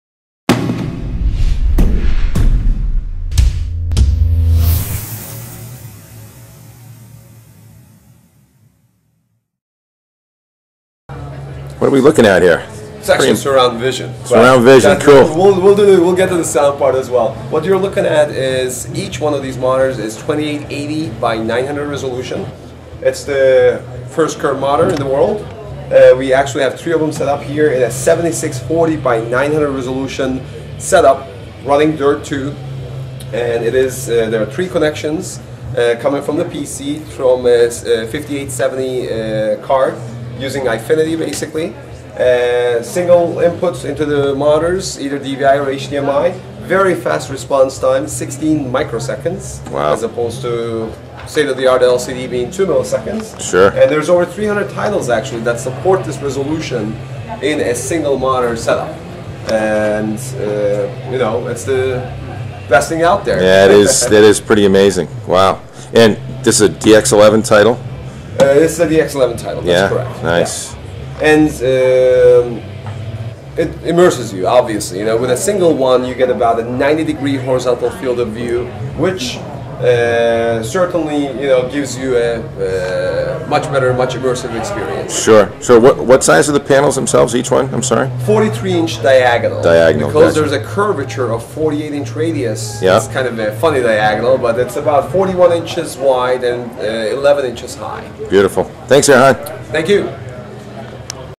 Ostendo AMD Eyefinity Surround Vision Gaming Demo - HotHardware
Though Ostendo CRVD panels have been around for a while now, in the upper press briefing rooms of AMD's spread at CES, Ostendo had married not one but three of their CRVD Curved Displays up with a proverbial bad-ass gaming system built in a Thermaltake Level 10 case.  Employing AMD Eyefinity technology with a Radeon HD 5870 graphics card pushing the pixels, the effect was completely "immersive" in the words of Ostendo Director of Marketing, Erhan Ercan.  We'd have to agree, hit the play below button and kick your feet up...